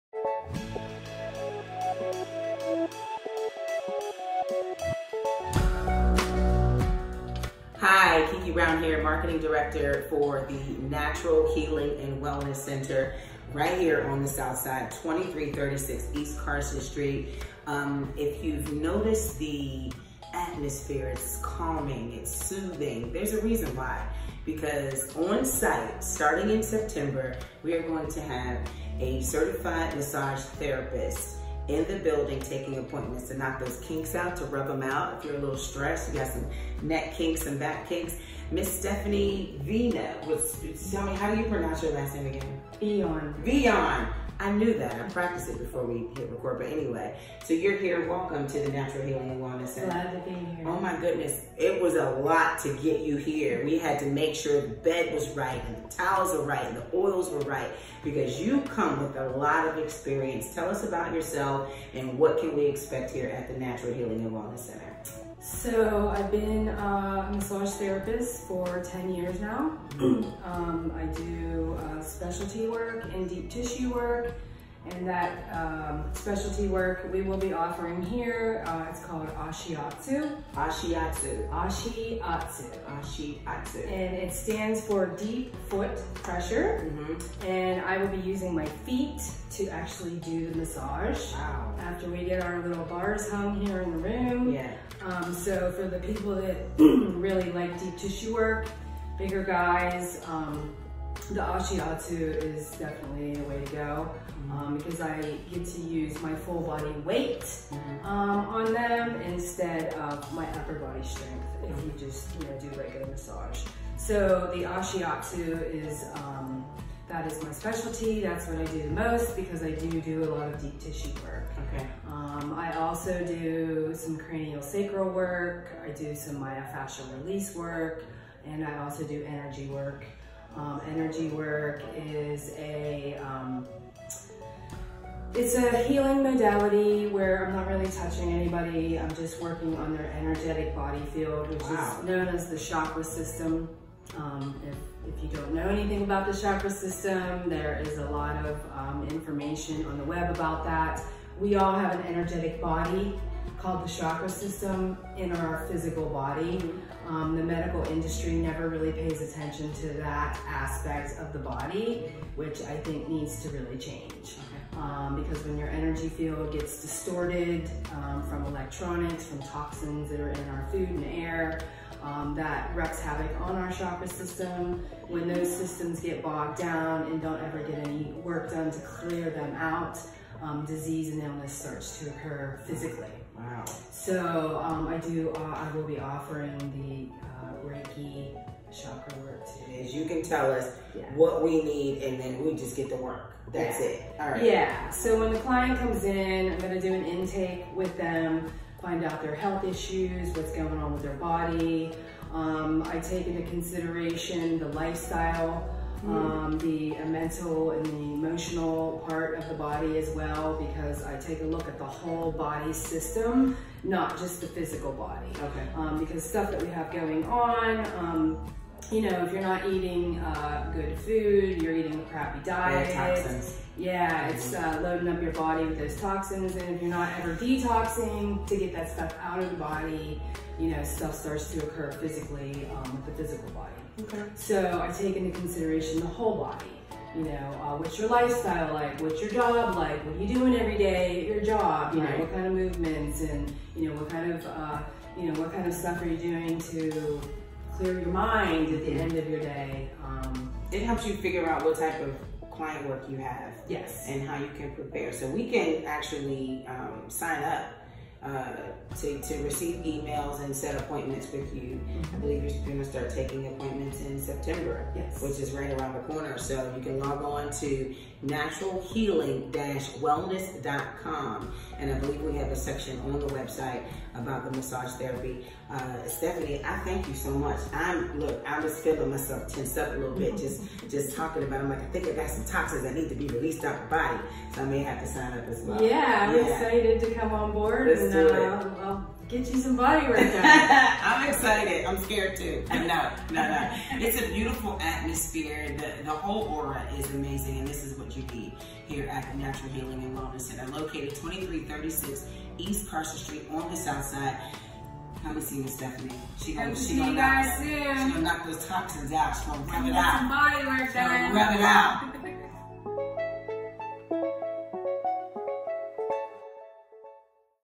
Natural Healing and Wellness Center Promo 3: Massage Therapist
Stop by the Natural Healing and Wellness Center and sign up for massage therapy service. Log onto for more information. (2336 East Carson Street on the Southside)